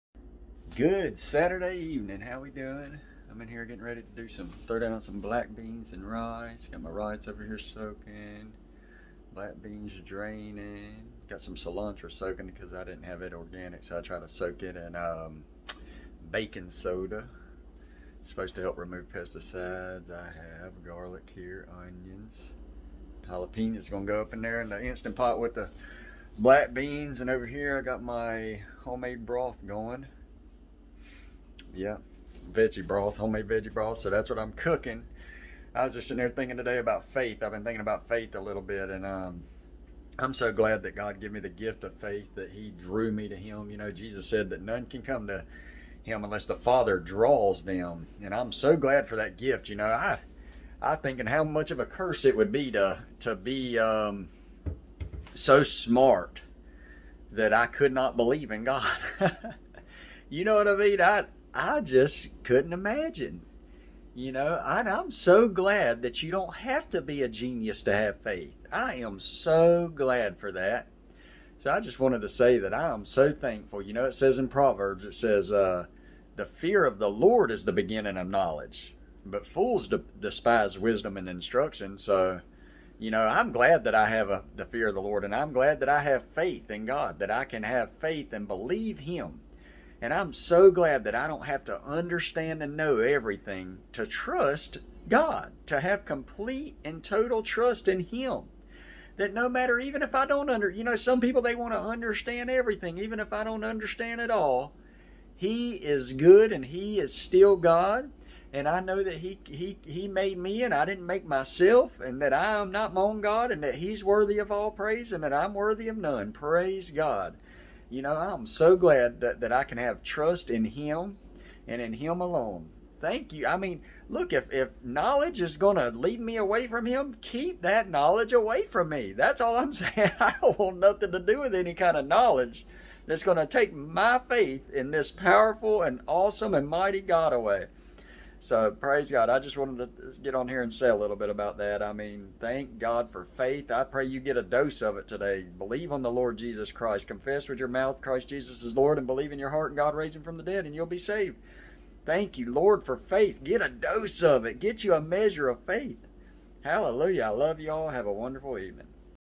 I'm so glad not to feel like I'm so "smart" or "high and mighty" that I don't believe in God.
Thank You Father that I'm not so "smart" that I don't have faith to believe in You and Your Son Jesus Christ! The fear of the LORD is the beginning of knowledge! God bless. Please send any prayer requests!